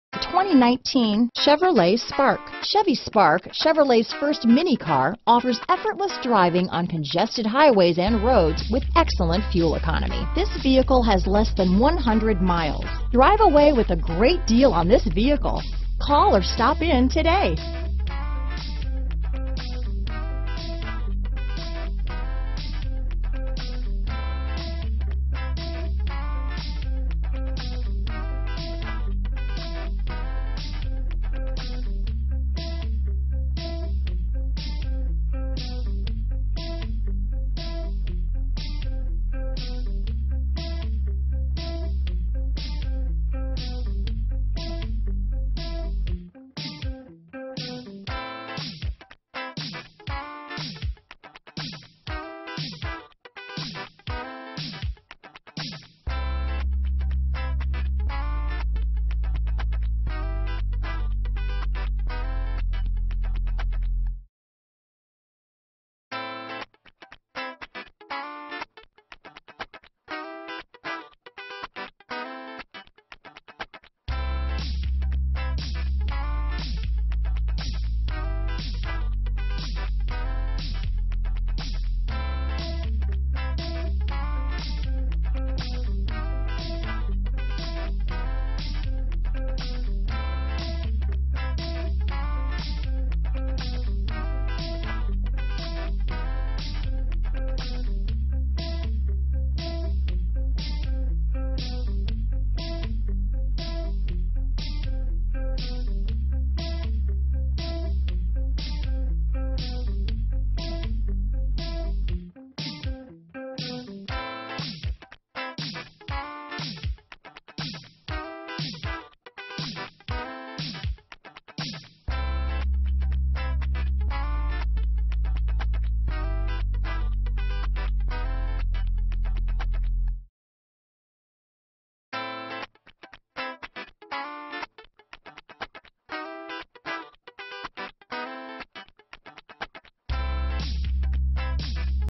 2019 Chevrolet Spark Brownsville, McAllen, Harlingen, Rio Grande City, Raymonville, TX KC765502
Silver Ice Metallic  2019 Chevrolet Spark available in Brownsville, Texas at Tipotex Chevrolet. Servicing the McAllen, Harlingen, Rio Grande City, Raymonville, TX area.

Tipotex Chevrolet
1600 Expy 77

The 2019 Chevrolet Spark is a small wonder. Bold design and vibrant new colors make the Spark the subcompact car thats easy on the eyes and hard to ignore. Nimble handling and an efficient, EPA-estimated 38 MPG highway and its 1.4L engine make this fun-size car perfect for hitting the open road. The Spark offers a range of convenient features for staying connected on the road, including built-in Wi-Fi, 7in color touchscreen infotainment display, Apple CarPlay and Android Auto compatibility, 2 USB ports and Chevrolet Connected Access and myChevrolet app access. Youll also find a comprehensive suite of safety features such as low speed forward automatic braking that will trigger when the system detects a front end collision between 5 and 37mph, forward collision alert, lane departure warning to prevent you from drifting out of your lane or merging at an inopportune time, rear park assist and a standard rear vision camera that displays the rear view on the infotainment touchscreen. The 2019 Chevrolet Spark is here to help you stay safe, plugged in and turning heads on your next adventure.